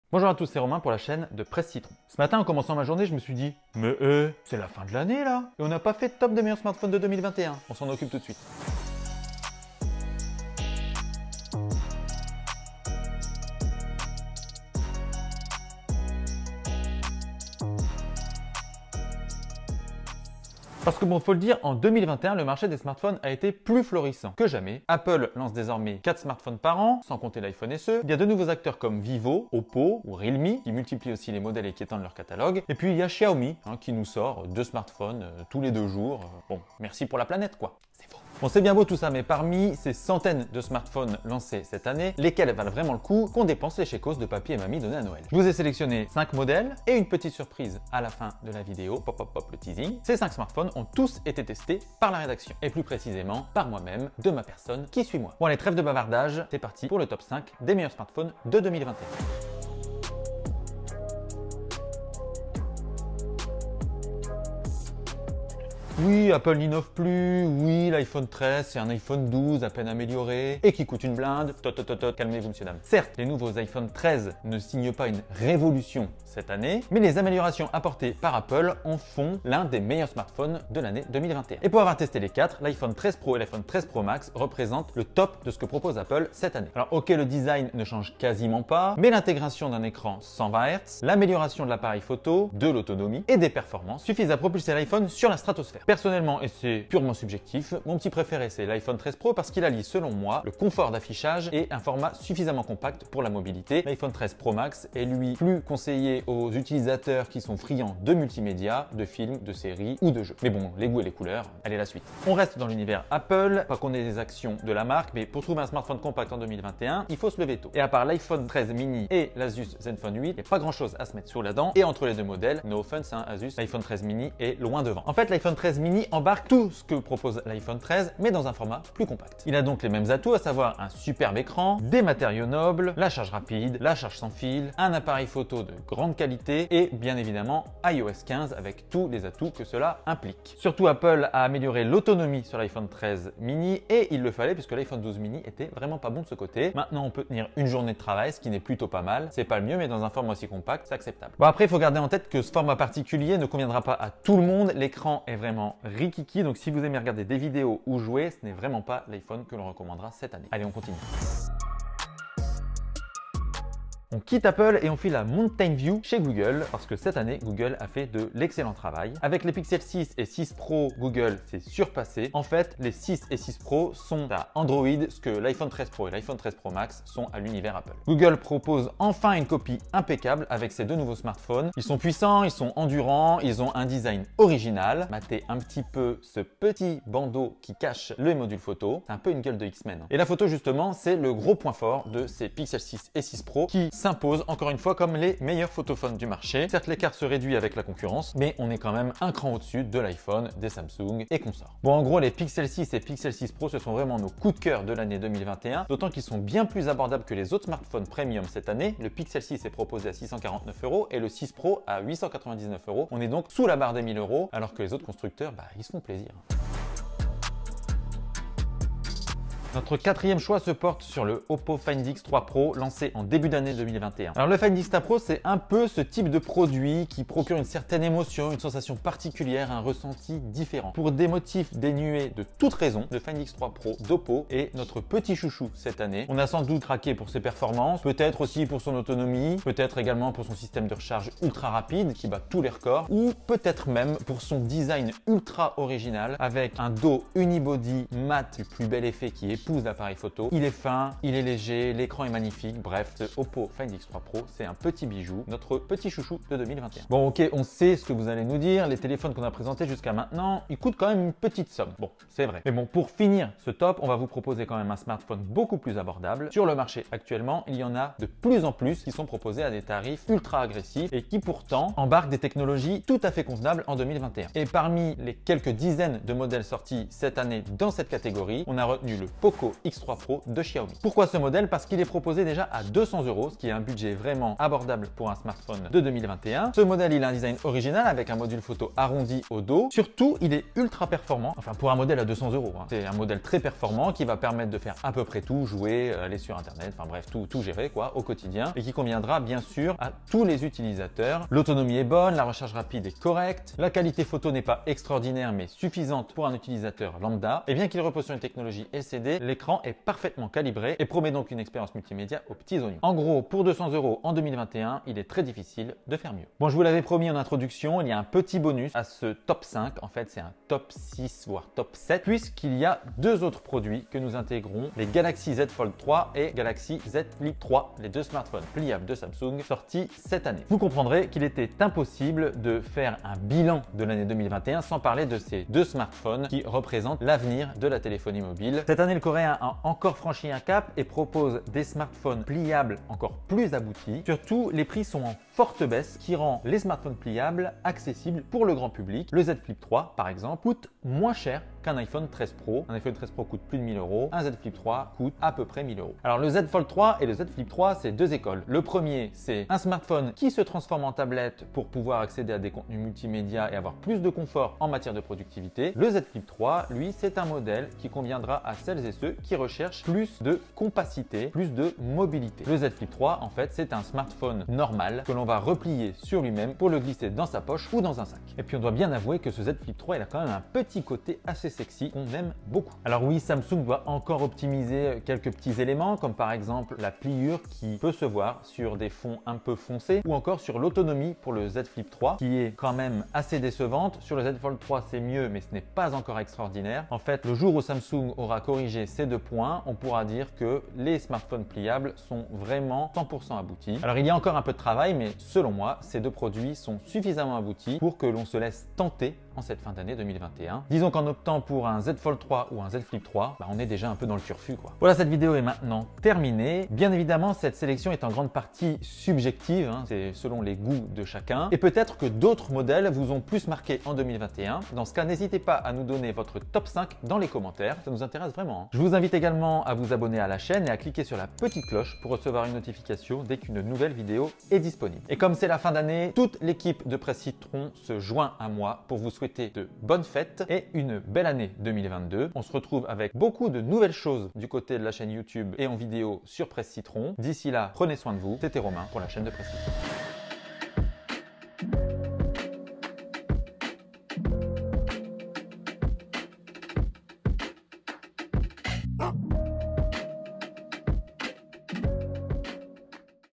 TOP 5 des MEILLEURS smartphones de 2021
Et si on faisait un petit bilan des meilleurs smartphones de l’année ? Voici le TOP5 de 2021 !

00:00 Introduction
00:58 iPhone 13 Pro & 13 Mini
02:41 Google Pixel 6 & 6 Pro
03:36 Oppo Find X3 Pro
04:40 Poco X3 Pro
05:24 Samsung Galaxy Z Fold3 & Z Flip3
06:55 Outro

📹 Présenté par Romain Vitt
🎬 Monté par Rémi Lou
🎧 Musique outro by Snooz